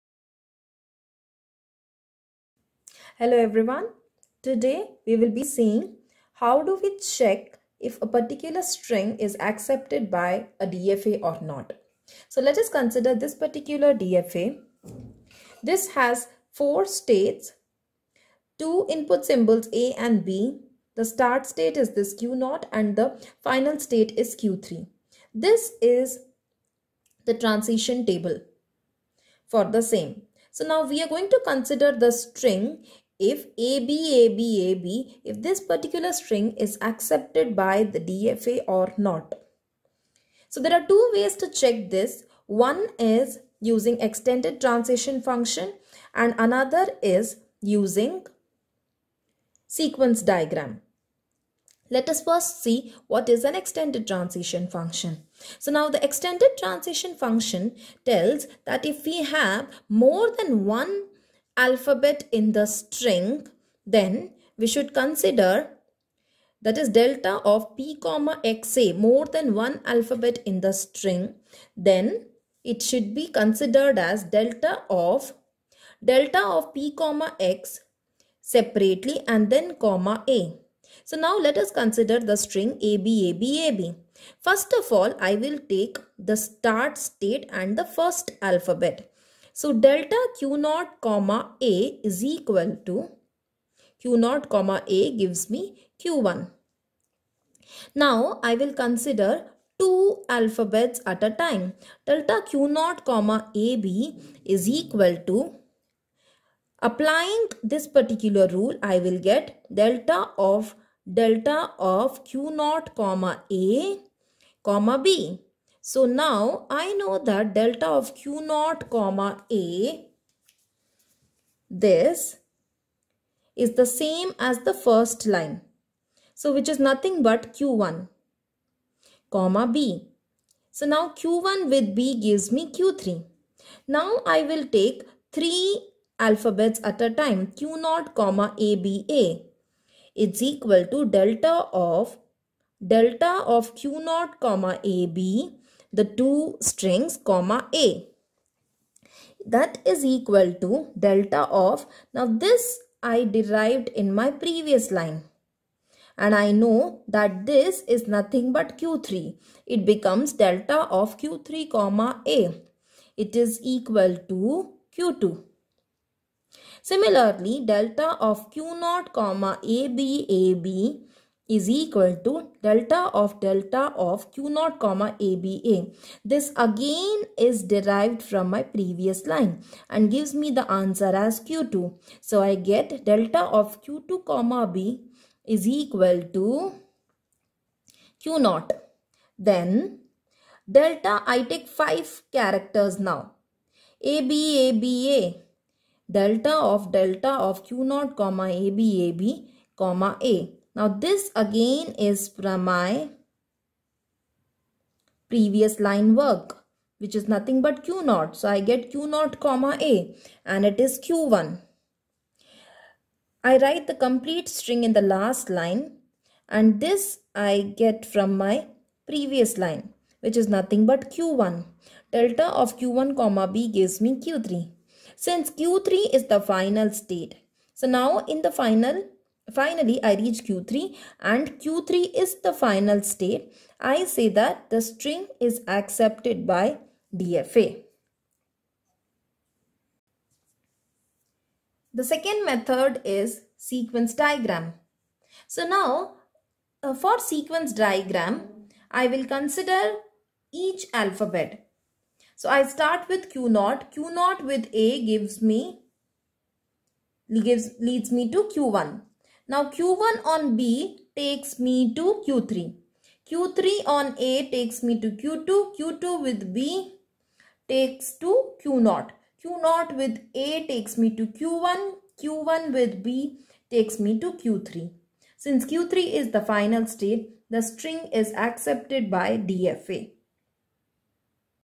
DFA- Checking if string is accepted by DFA or not| Deterministic Finite Automata| Lec 02
The video includes lecture on checking if string is accepted by DFA or not. Browse through for more content!